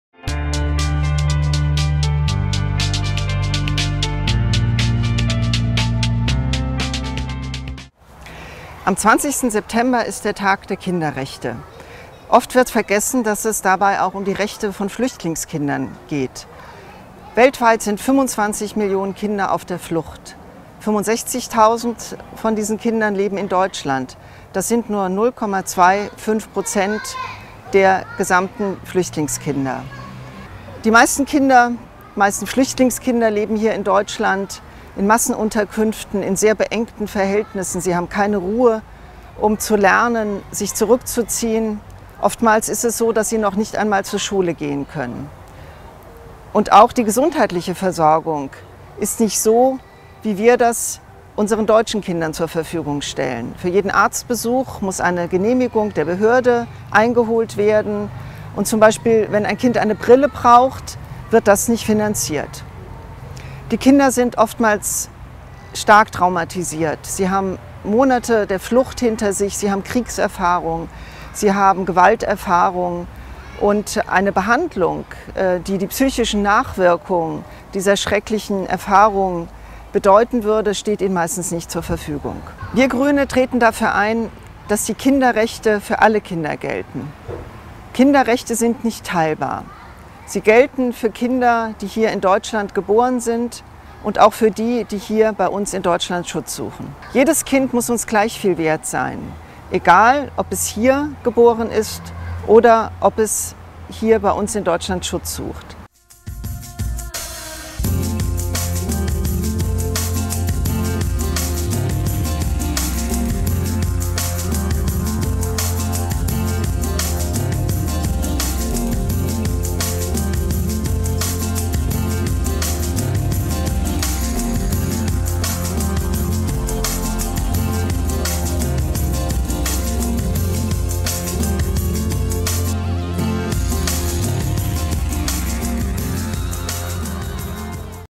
Weltkindertag 2014: Andrea Asch MdL über die Rechte von Flüchtlingskindern
25 Jahre Kinderrechte: Der Weltkindertag 2014 steht unter dem Motto "Jedes Kind hat Rechte". Andrea Asch MdL macht in diesem Video besonders auf die Rechte von Flüchtlingskindern aufmerksam. Diese haben nämlich oft nicht dieselben Rechte wie deutsche Kinder, obwohl Deutschland die UN-Kinderrechtskonvention unterschrieben hat.